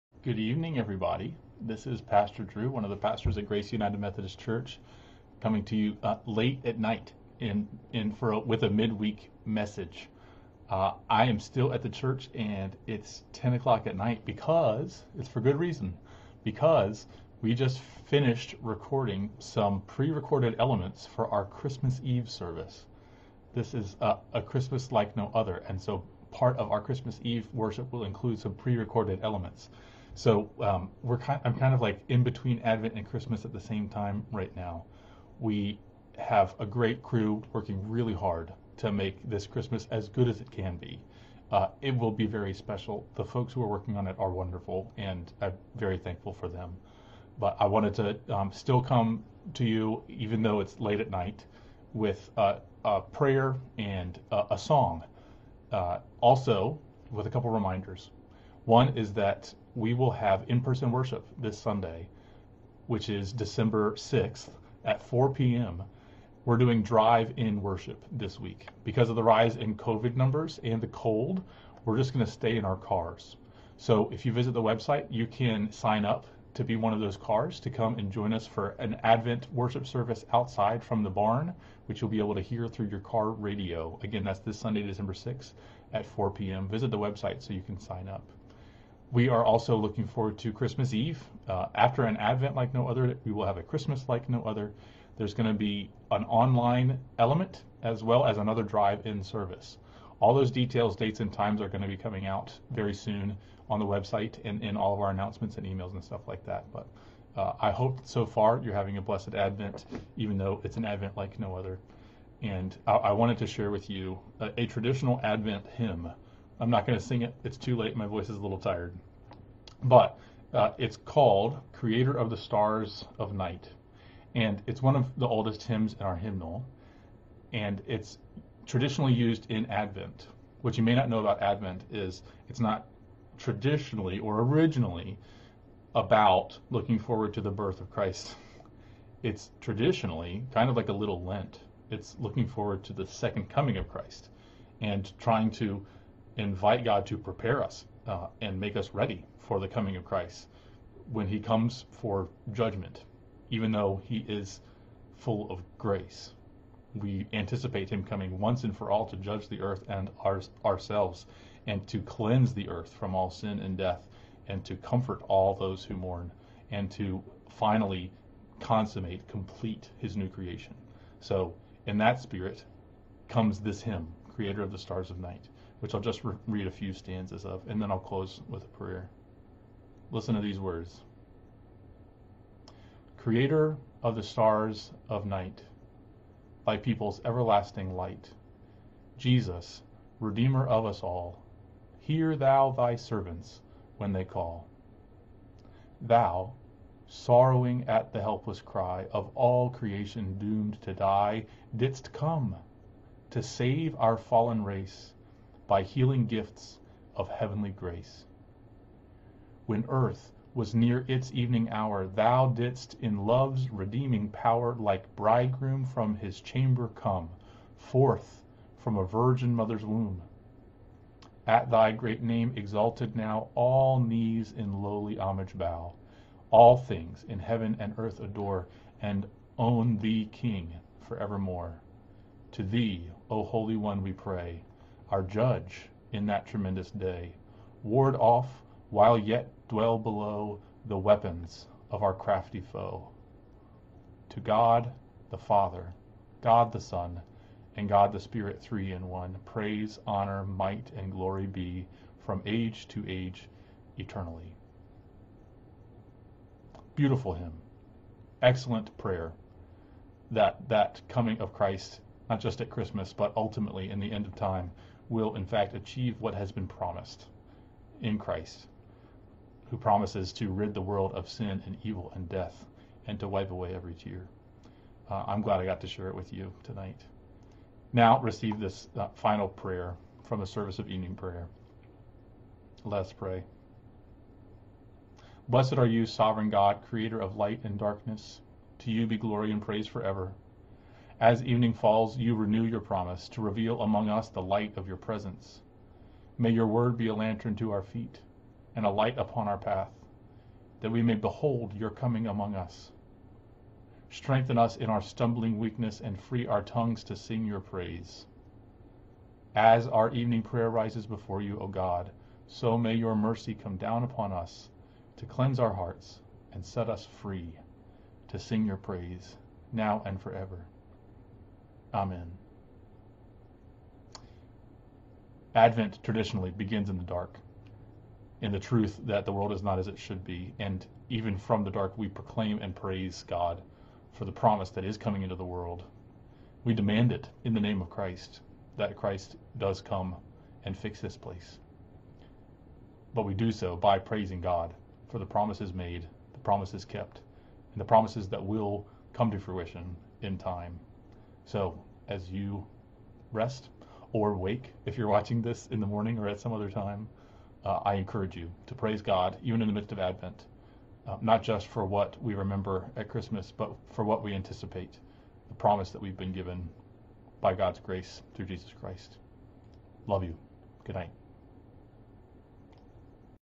Mid-week Message - Drive in worship & Christmas Eve worship updates!
A few late night notes (plus a hymn and a prayer) from Pastor Drew:
1. Were looking forward to a blessed Advent and Christmas like no other.
2. In Person (Drive-in) Worship this Sunday!
3. Christmas Eve will be online and in-person (Drive-in)